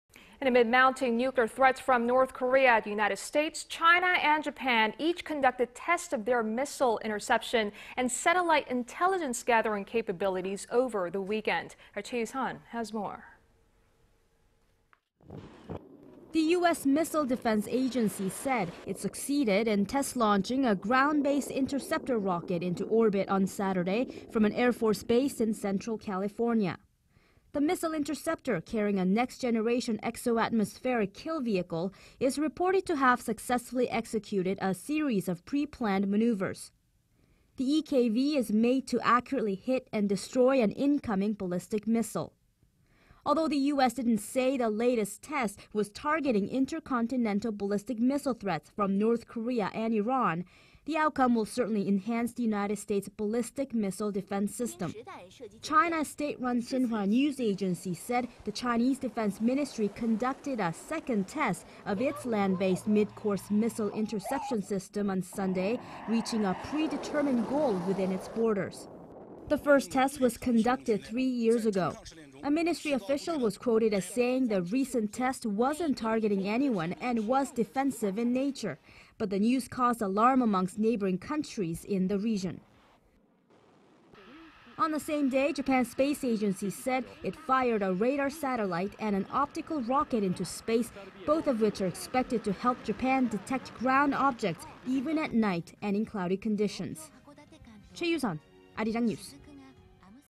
U.S., China, Japan Test Missile Inteception, Improve Intelligence Gathering [Arirang News]
Amid mounting nuclear threats from North Korea... the United States, China and Japan each conducted tests of their

missile interception and satellite intelligence gathering capabilities over the weekend.Choi You-sun has more.

he U.S. Missile Defense Agency said it succeeded in test-launching a Ground-Based Interceptor rocket into orbit on

Saturday, from an air force base in central California.

The missile interceptor, carrying a next-generation Exoatmospheric Kill Vehicle, is reported to have successfully executed

a series of pre-planned maneuvers.The EKV is made to accurately hit and destroy an incoming ballistic missile.
Although the U.S. didn't say the latest test was targeting intercontinental ballistic missile threats from North Korea and

Iran, the outcome will certainly enhance the United States' ballistic missile defense system.

China's state-run Xinhua News Agency said, the Chinese defense ministry conducted a second test of its land-based

mid-course missile interception system on Sunday, reaching a pre-determined goal within its borders. The first test was

conducted three years ago.

A ministry official was quoted as saying the recent test wasn't targeting anyone and was defensive in nature, but the

news caused alarm amongst neighboring countries in the region.

On the same day, Japan's space agency said it fired a radar satellite and an optical rocket into space, both of which are

expected to help Japan detect ground objects even at night and in cloudy conditions.

Choi You-sun, Arirang News.